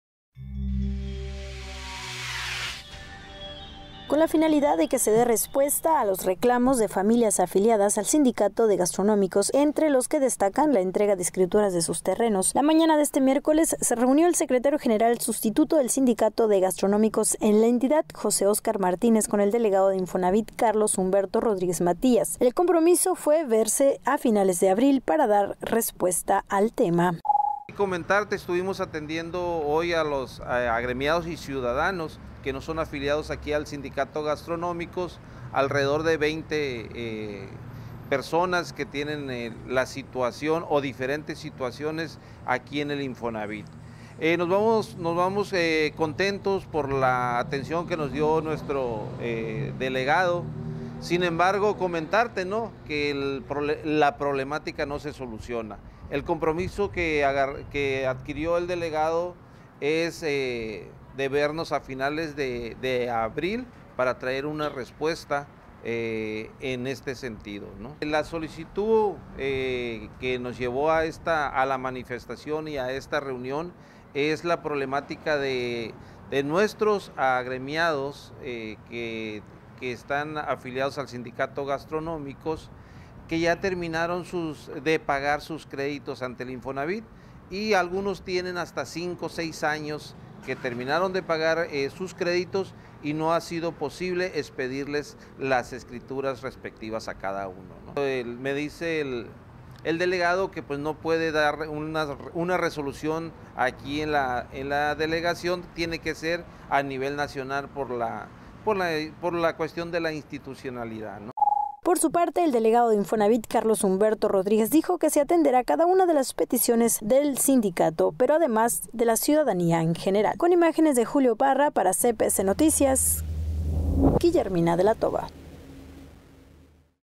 Acuerda sindicato de Gastronómicos agilizar entrega de escrituras
Acuerda Sindicato de Gastronómicos con Delegado de Infonavit agilizar la entrega de escrituras a quienes ya pagaron.